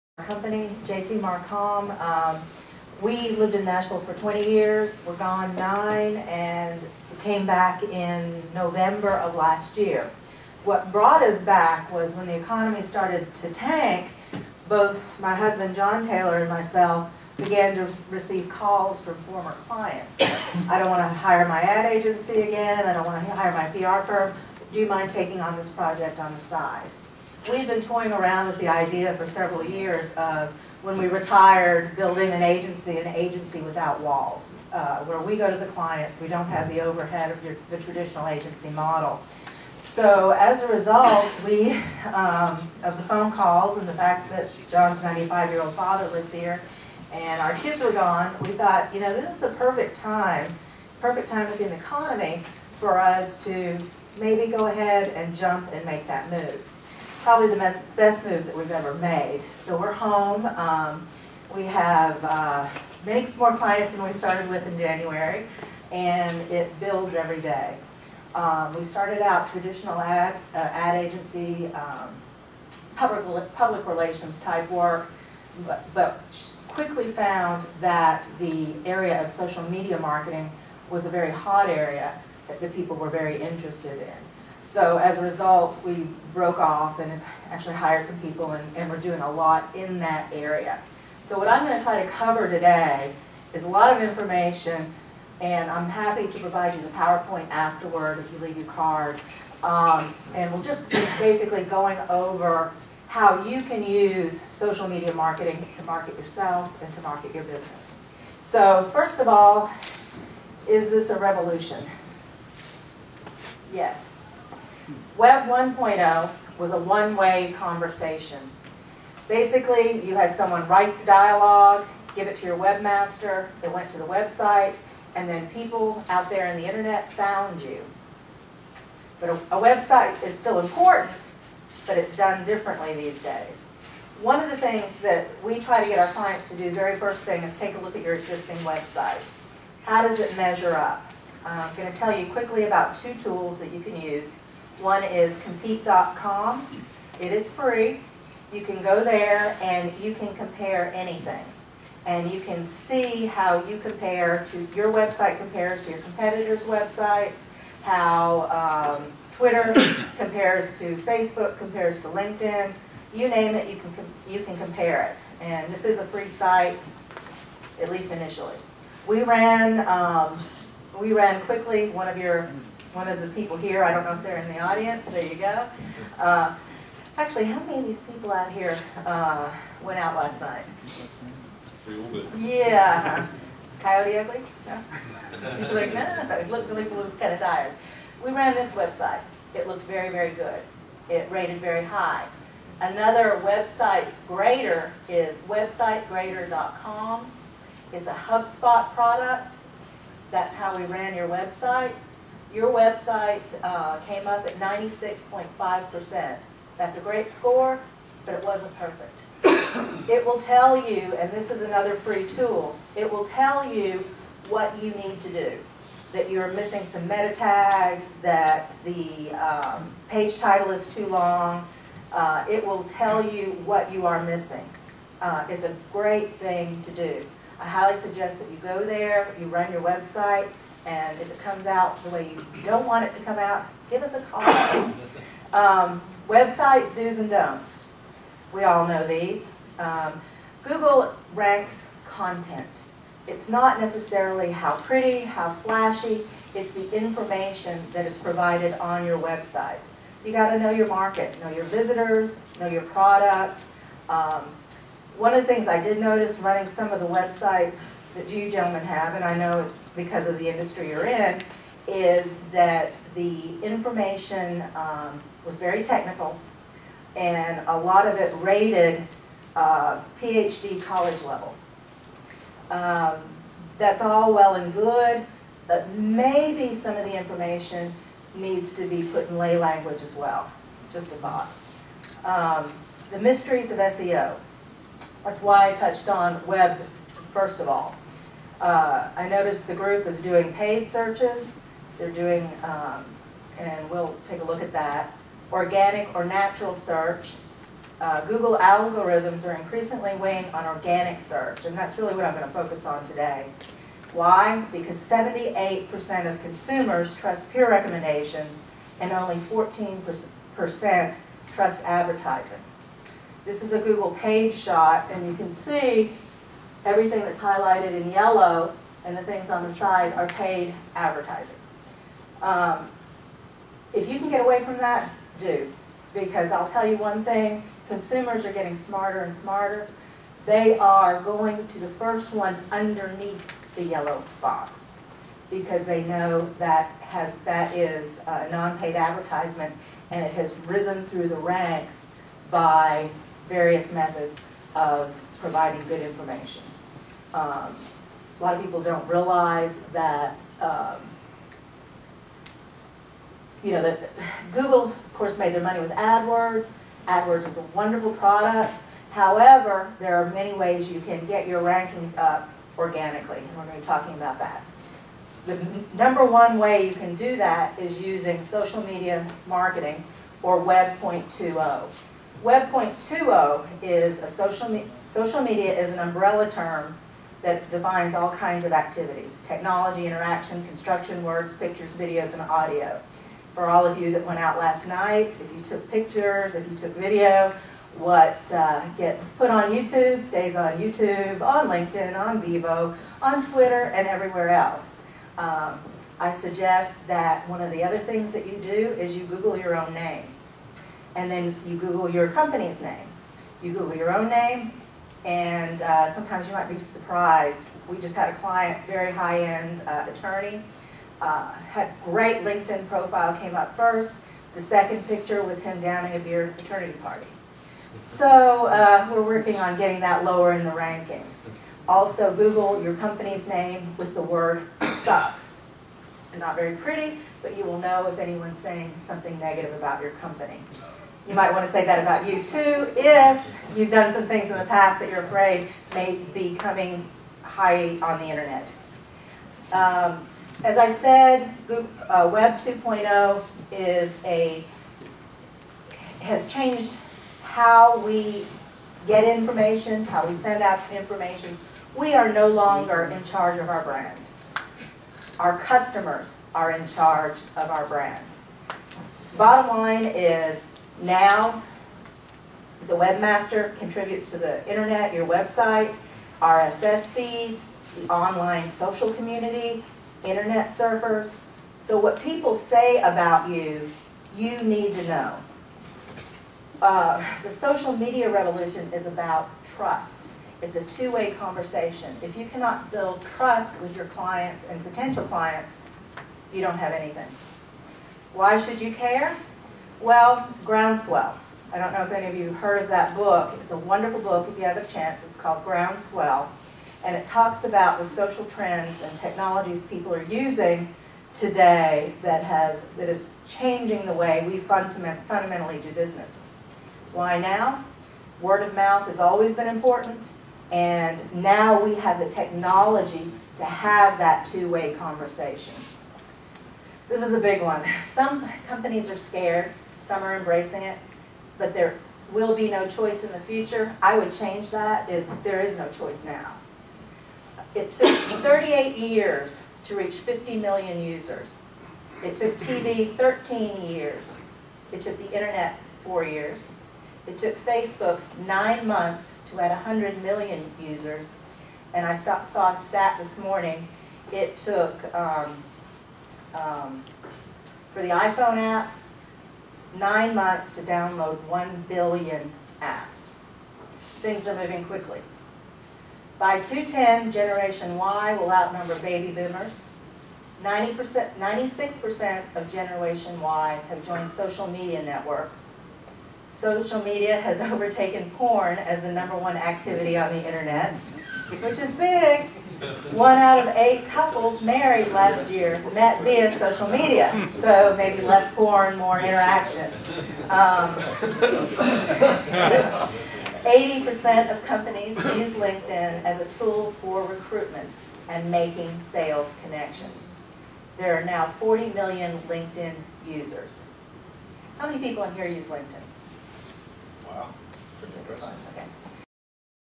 BareMetal Data World 2009 : Christine Taylor
Internet Marketing - The Social Media Revolution by Christine Taylor - JTMarCom